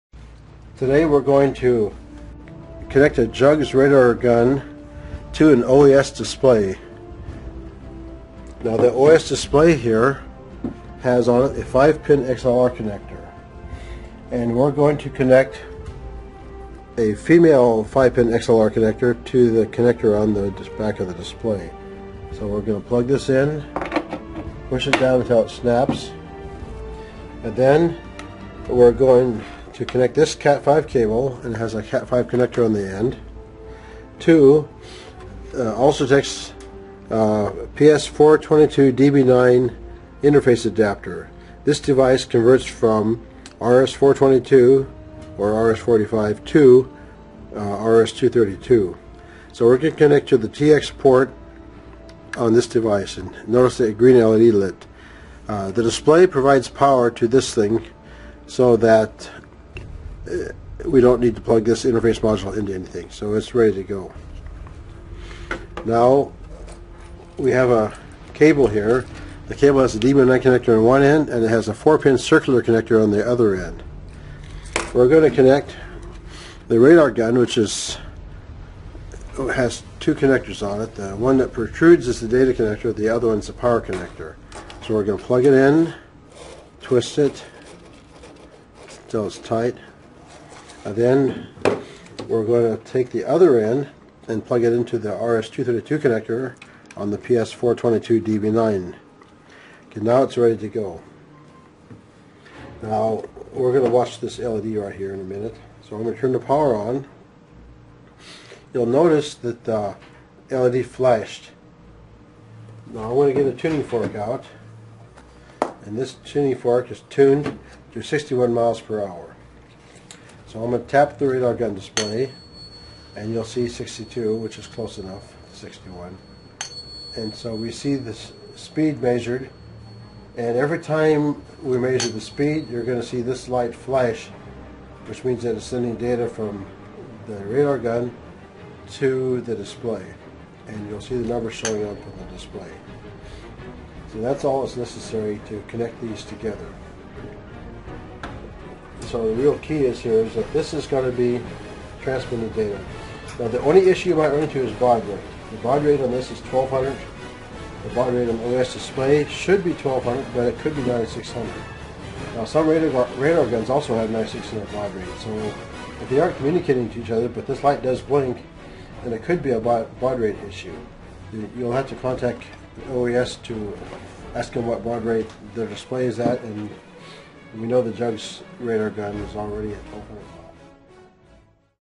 Alzatex Jugs Radar Gun
Alzatex Jugs Radar Gun troubleshooting guide. Most of the procedures in this video will require someone with some technical and/or computer experience. The visual inspection test can be performed by almost anyone. The continuity and voltage tests may require someone with electrical or electronics experience. The data communications tests will require someone that has computer experience. for more information.